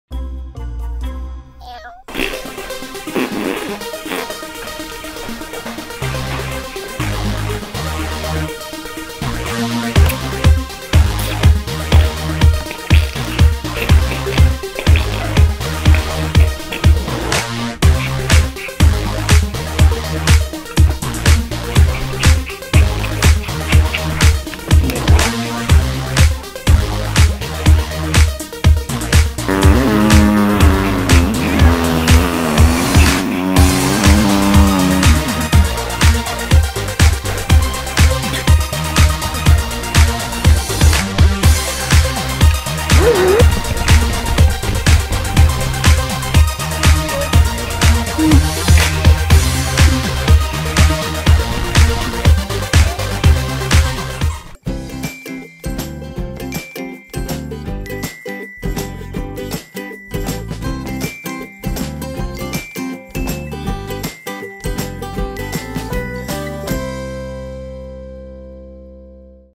Funniest Pug Puppy Wears Onesie With Footies, Not Convinced
He doesn't want to admit how awesome he looks.
Follow Frankie on Instagram @frankiespuglife